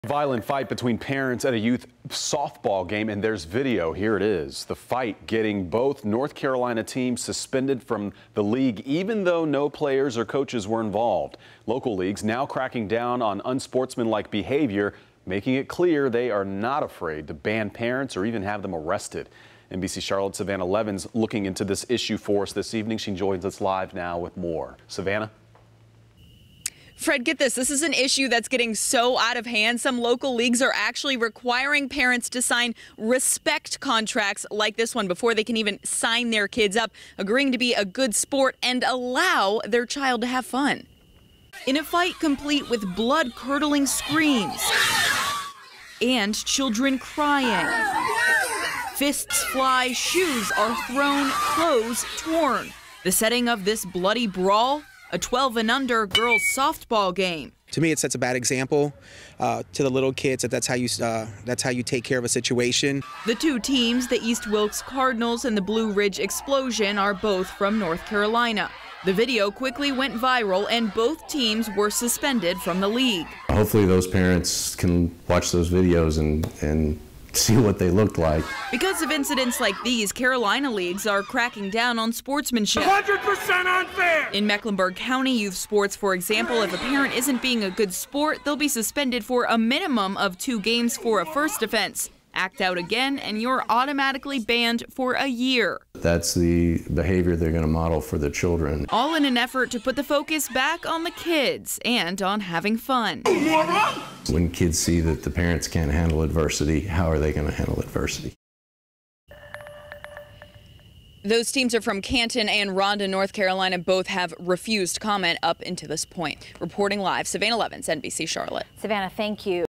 Parents brawl during youth softball tournament
A video shows more than a half-dozen fans tackling each other to the ground amid a flurry of punches and kicks.